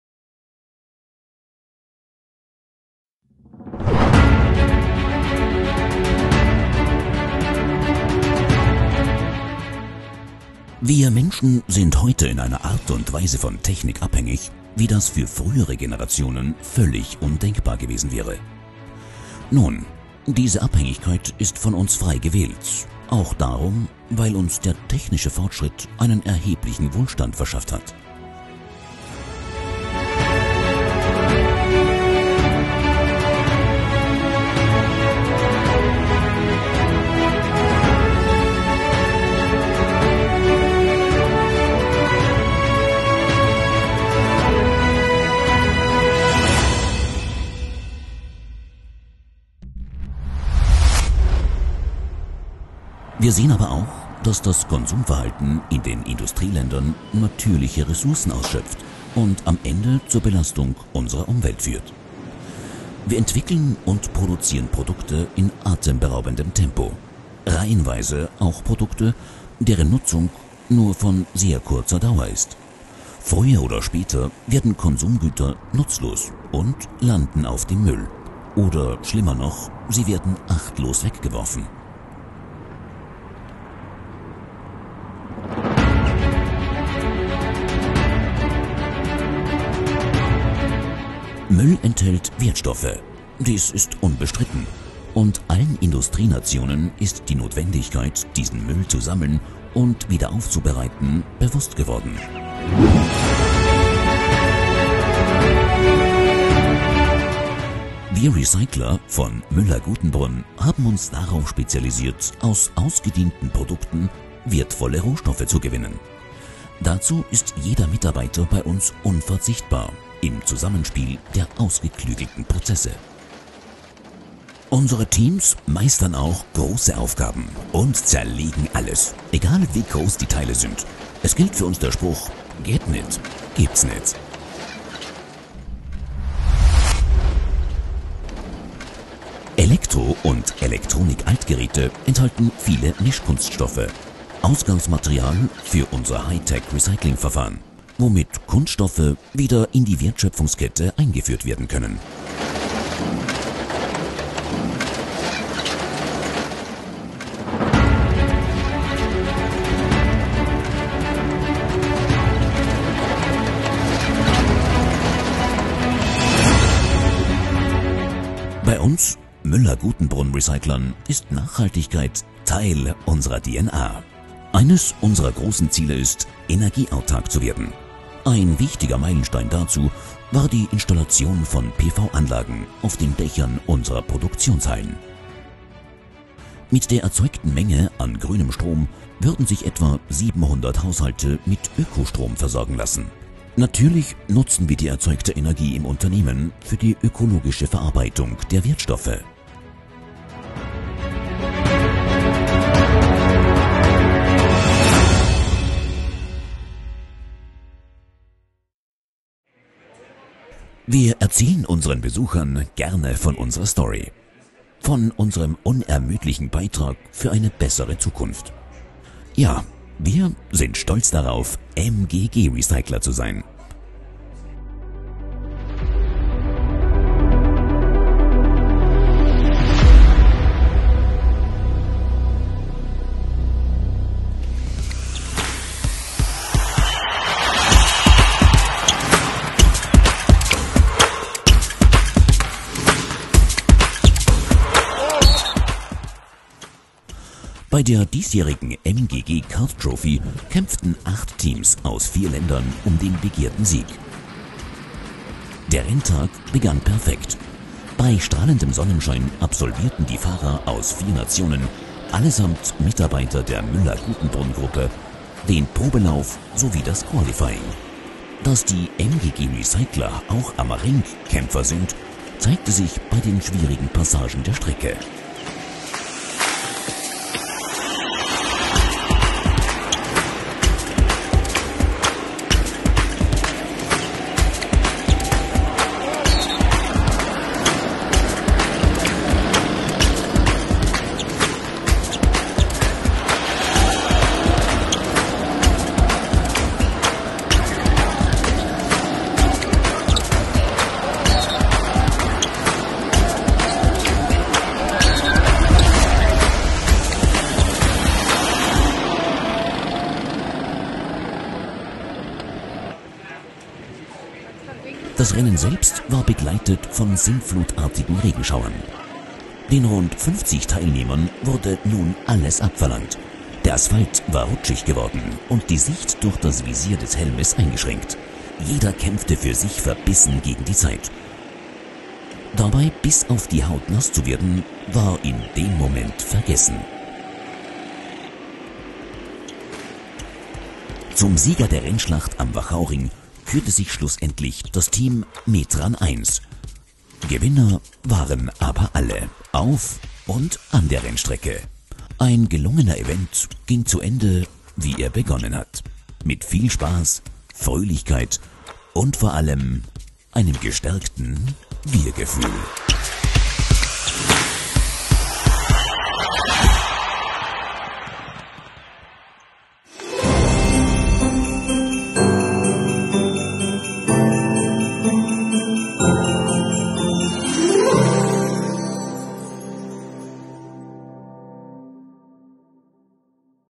Recycling beginnt daheim :-)
Vertonung des Videos für die Allround-Recycler Müller-Guttenbrunn Group | Amstetten.

Ein Danke auch an für die perfekte Umsetzung.